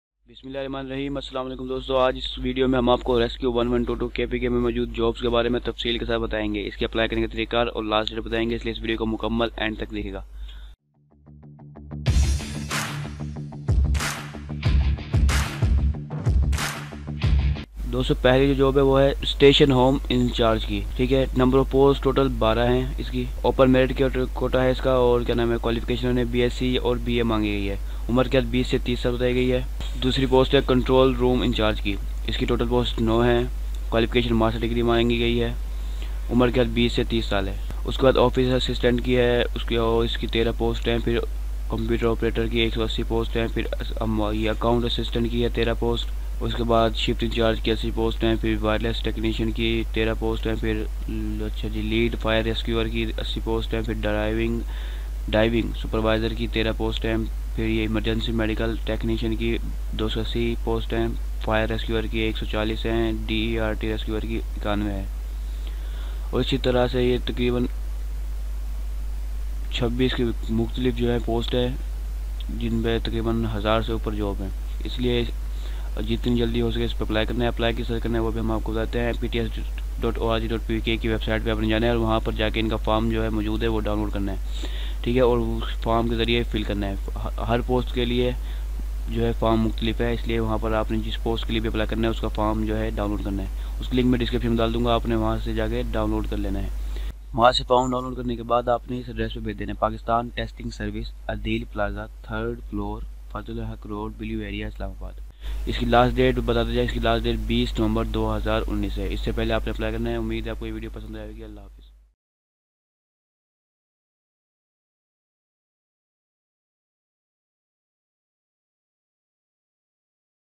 Rescue 1122 KPK Latest Jobs 2019
AboutJobb Rescue 1122 KPK Latest Jobs 2019
JobsInPakistan2019 Jobs in Pakistan 2019
- - - - - - - - - - - - - - - - - - - - - - - - - - Link Below- - - - - - - - - - - - - - - - - - - - - -

Rescue 1122 Jobs 2019: Rescue 1122 is inviting applications from eligible candidates for 1796+ Computer Operators, Emergency Medical Technicians, Assistants, Incharge, Clerks, Drivers & Other vacancies in Khyber Pakhtunkhwa newly merged districts. PTS is conducting recruitment test for these posts and you can download the PTS application form and deposit slip from PTS website or this page below – Required qualification from a recognized institution, relevant work experience and age limit requirement are as following. Eligible candidates are encouraged to apply to the post in prescribed manner. Incomplete, late, hand written submissions/applications will not be entertained. Only shortlisted candidates will be called for written test/interview. See the notification below to see vacancies/positions available, eligibility criteria and other requirements. Eligible candidates must submit applications on or before 20th November 2019.